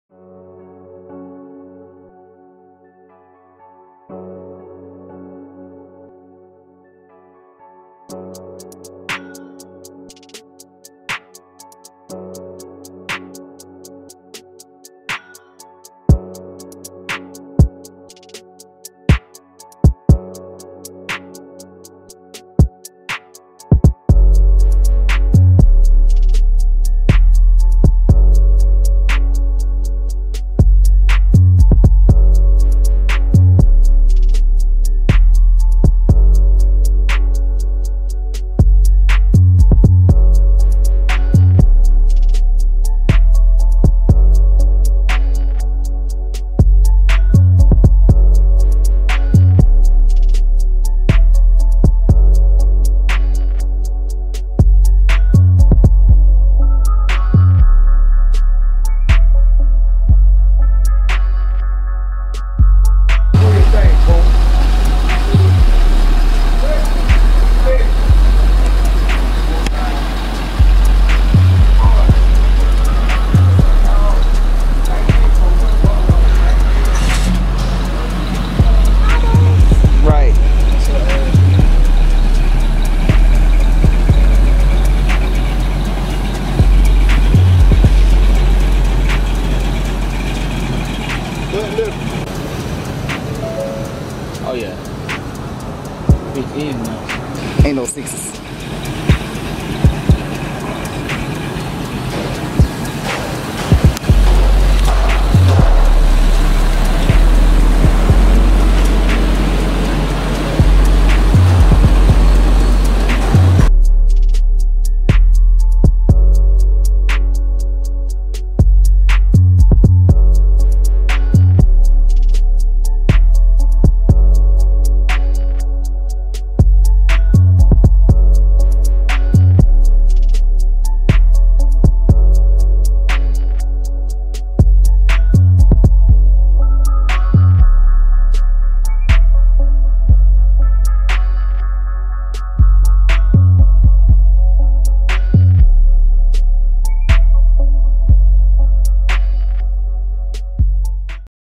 WhipAddict: $120k on 2019 Ford F350 King Ranch on 28" Forgiato Canale Dually
Brand new F350 Ford King Ranch FX4 Dually Truck straight off the lot to @CollegeParkCustoms for at brand new set of chromeForgiato Canale Duro 28s wrapped in 28" Fuel Tires.